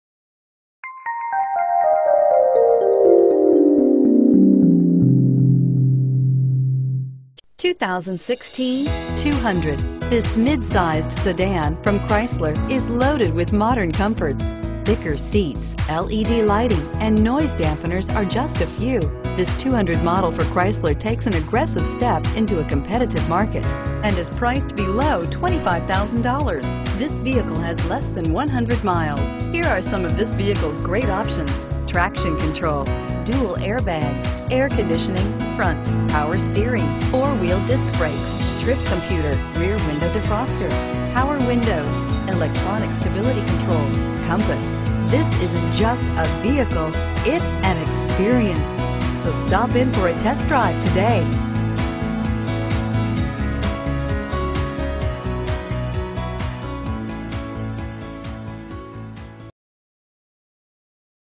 2016 Chrysler 200 Richardson TX GN174057
2016 Chrysler 200 Limited

Richardson CDJR
1321 N Central Expressway

Don't miss this 2016 Chrysler 200. It's equipped with Automatic transmission and features a Black exterior. With 1 miles, You'll want to take this sedan home. Make a great choice today.